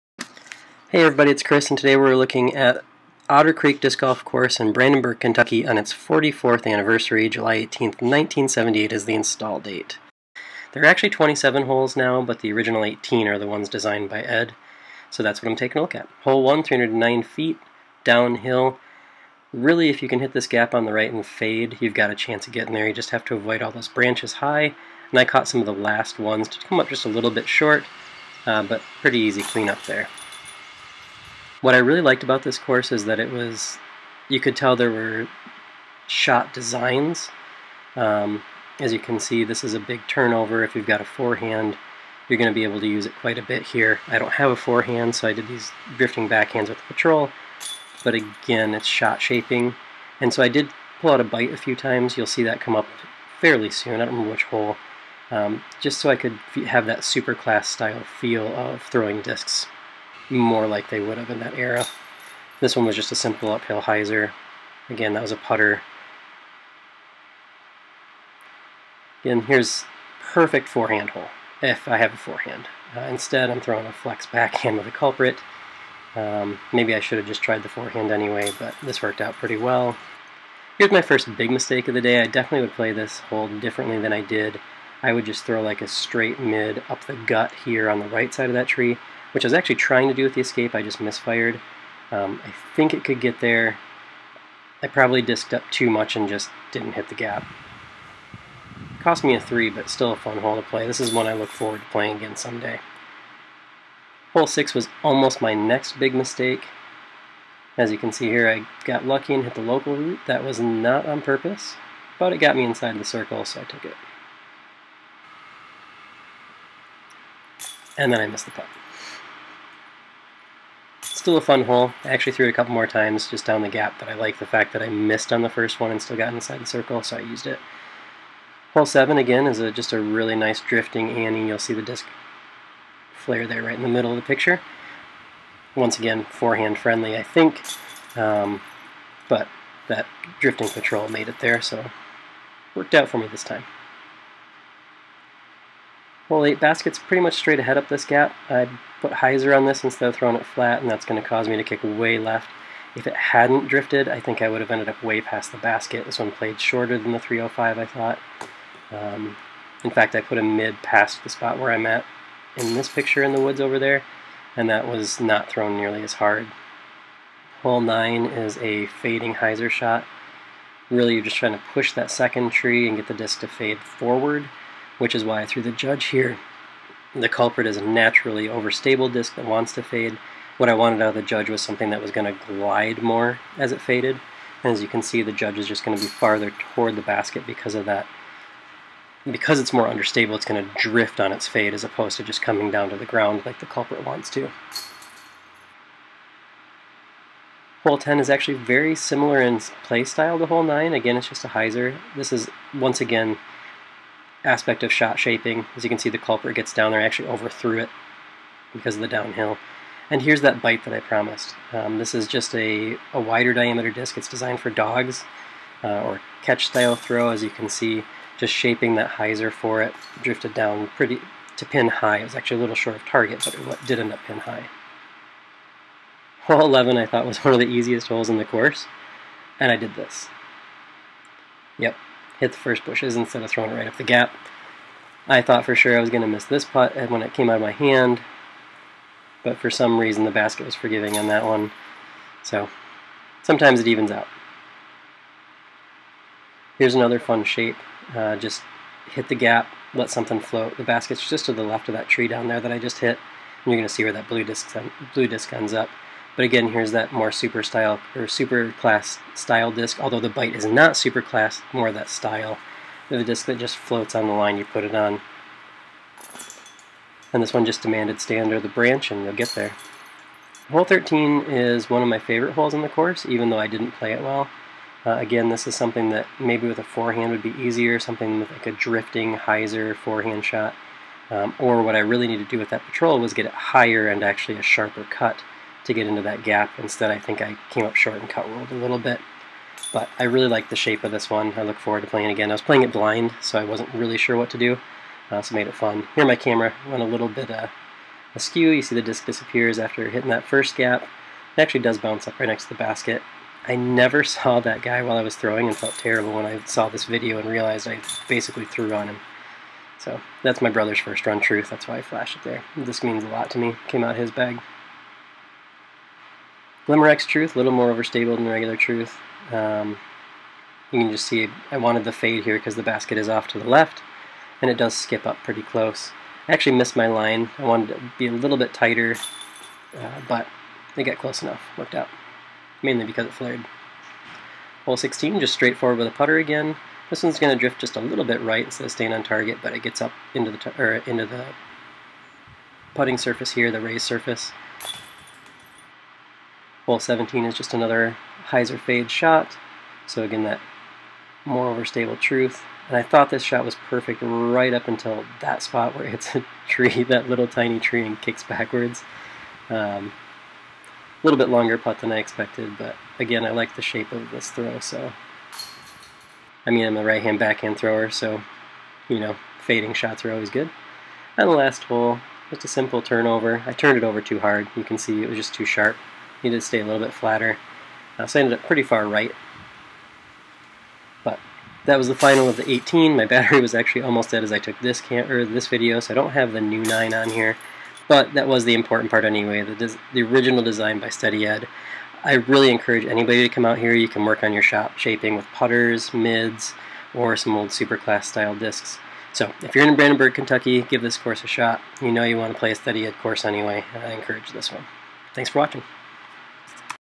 Otter Creek Disc Golf Park - Brandenburg, KY
Otter Creek DGP was the first disc golf course installed in the state of Kentucky. The course's initial 18 holes were designed by Steady Ed Headrick, making this course extra special. The original baskets are still in place, so expect old targets. The course is beginner accessible, while having some challenge left for experienced players looking to shape shots. I already look forward to my next trip to Otter. This video was filmed on the July 4th weekend, but I waited to release the footage for the course's 44th birthday today (according to the sign). This course is now 27 holes, but this video only includes the original 18.